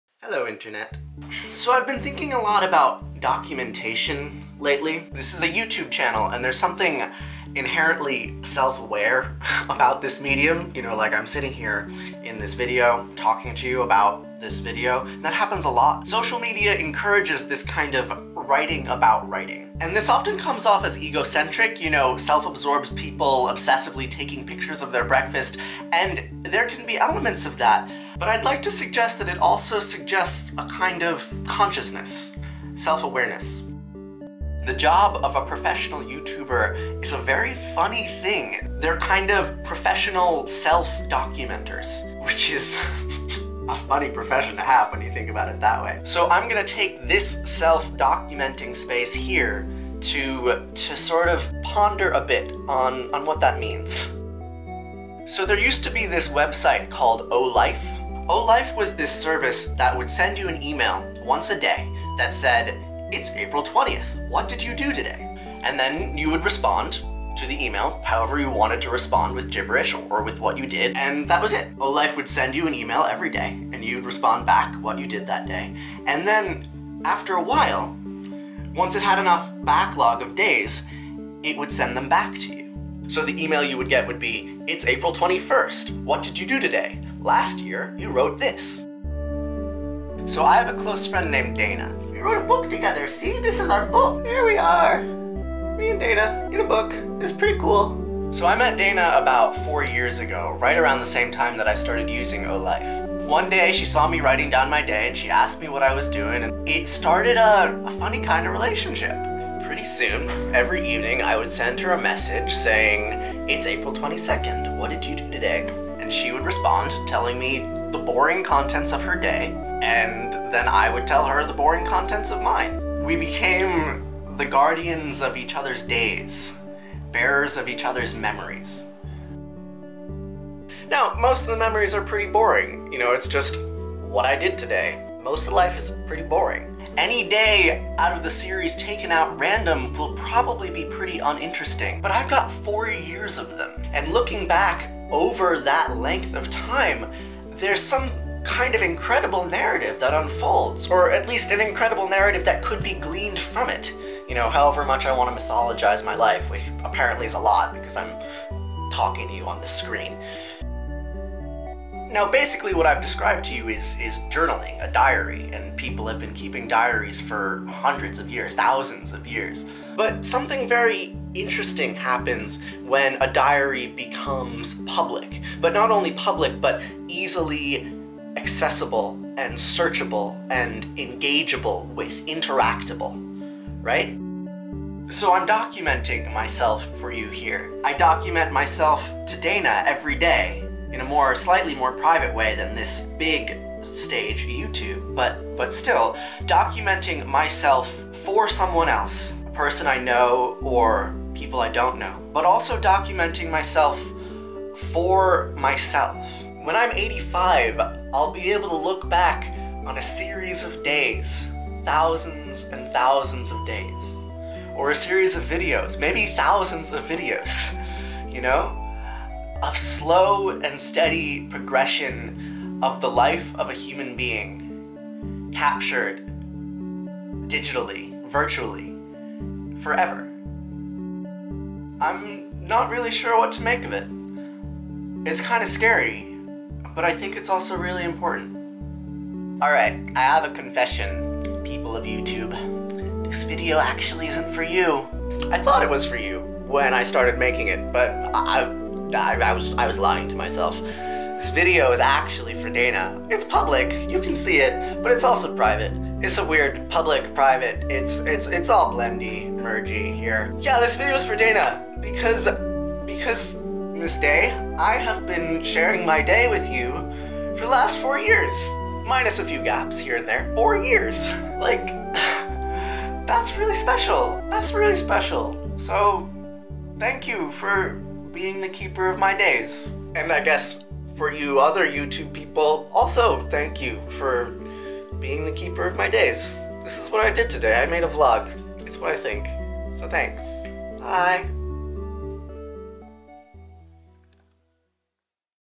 On the Remembering of Days
A video for my dear friend Dayna, in which Maximus talks about the practice of recording your day